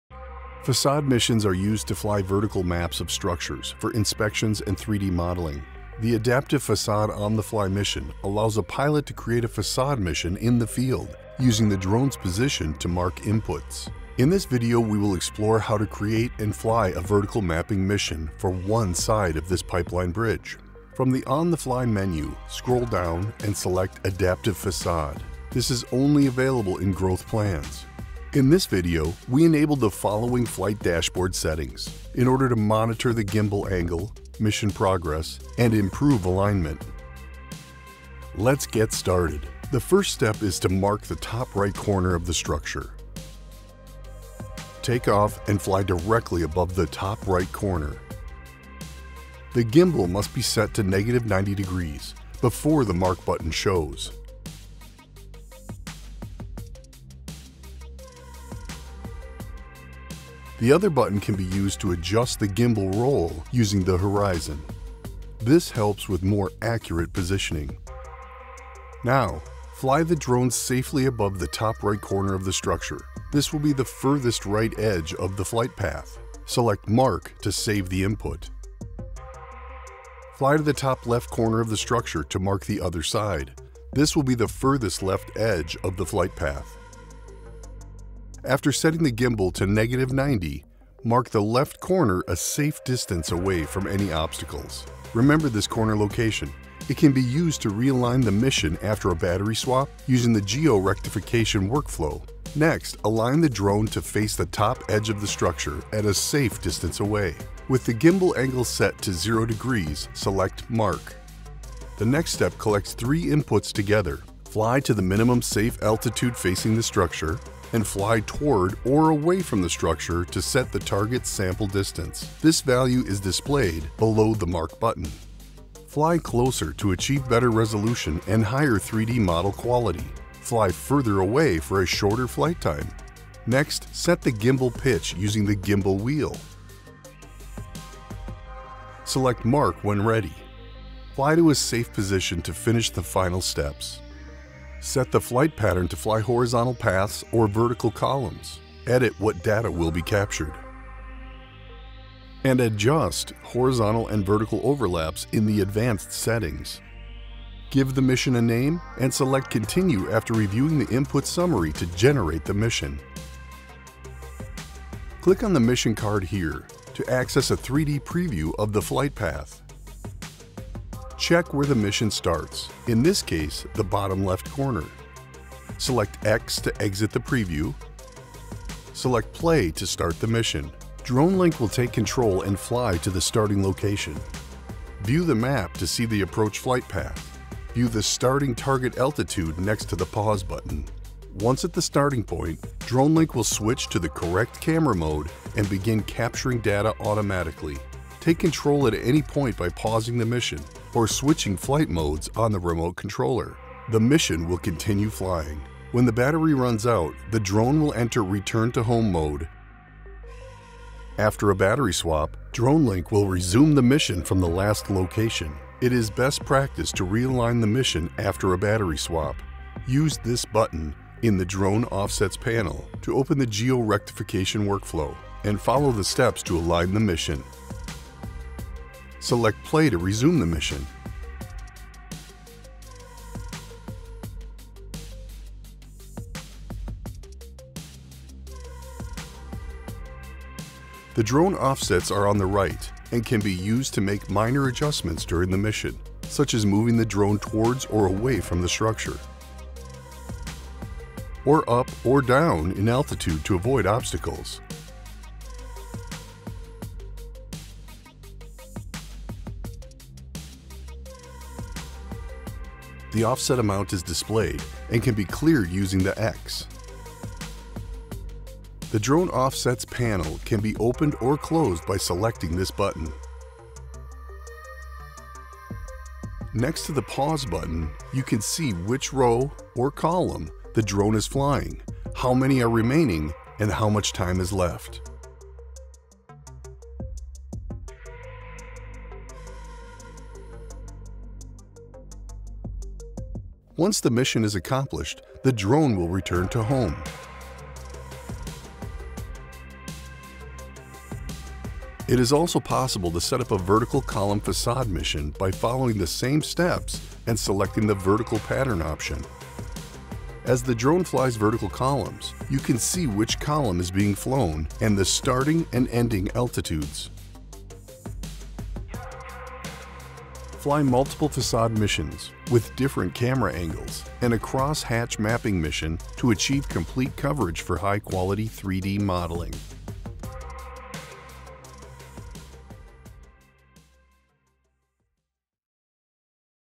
Facade Missions On-the-Fly (Adaptive Facade) - Vertical Mapping for Inspections and 3D Modeling
Learn how to create a vertical mapping mission of a structure using the Adaptive Facade On-the-Fly mission. Fly vertical columns or horizontal paths for inspections and 3D modeling. Set the boundary of the facade, vertical or horizontal overlap, target distance, and gimbal angle. Set up the entire mission in the field using the drones position. Make adjustments real-time with drone offsets.

Adaptive Facade missions require a Growth plan.
Try a Growth plan with our 14 day money back guarantee, and pause monthly subscriptions anytime.

00:00 - Intro
00:21 - Open Adaptive Facade On-the-Fly Mission
00:28 - Flight Dashboard Settings
00:39 - Mission Setup Step 1 - Right Corner
01:19 - Step 2 - Left Corner (Reference)
01:41 - Step 3 - Top Edge
01:51 - Step 4 - Top Edge
02:24 - Step 5 - Pattern Horizontal or Vertical
02:29 - Other Settings - Data Capture, Overlaps, Name
02:39 - Generate Mission and 3D Preview
02:58 - Start Mission
03:26 - Battery Swap RTH and Resuming
04:00 - Drone Offsets for Real-Time Adjustments
04:34 - Flying Horizontal Facade Mission
05:01 - Flying Vertical Facade Mission